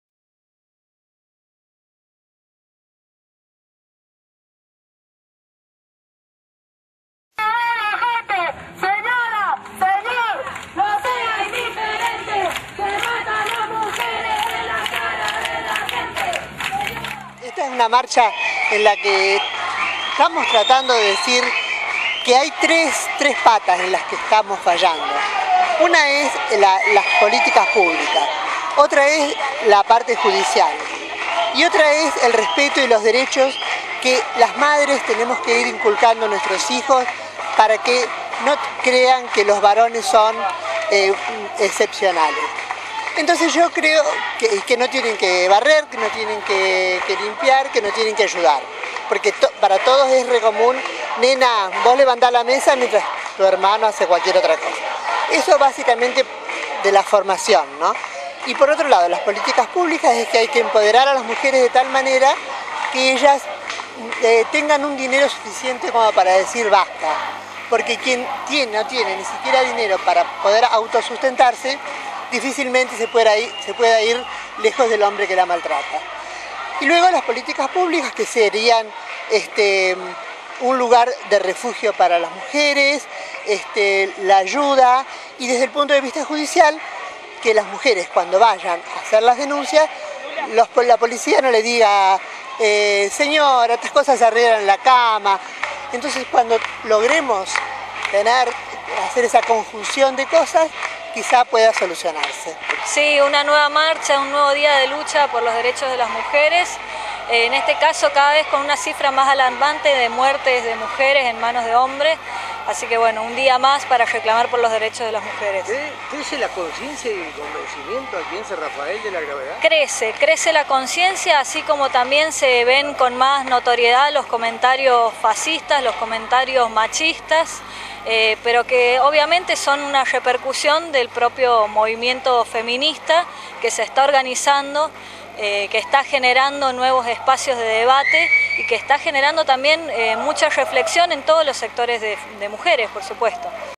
TODOS CONTRA LA VIOLENCIA DE GÉNERO
Instituciones, hombres y mujeres, también marcharon en San Rafael, adhirieron a los actos realizados en todo el mundo en conmemoración del Día Internacional de la Mujer